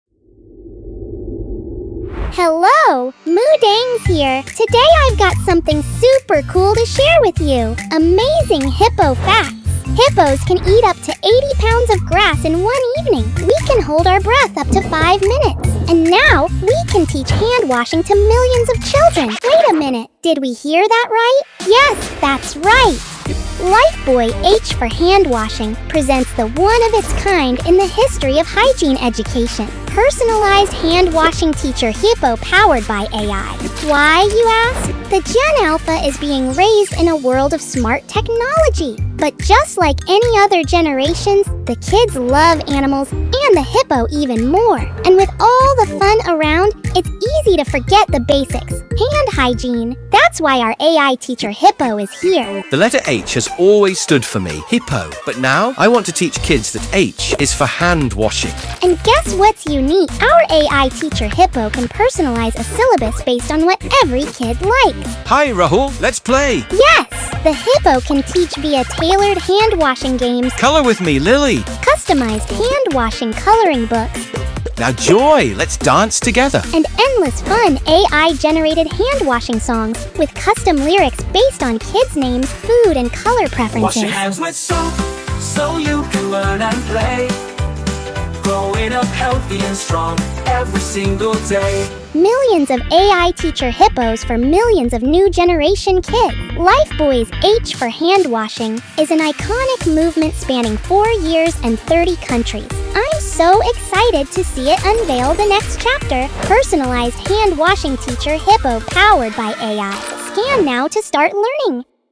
Learn H for Handwashing with your AI Teacher Hippo
This Global Handwashing Day Lifebuoy launches AI-Teacher Hippo, our latest H for Handwashing educational tool that combines AR+AI to personalize handwashing education for millions of kids.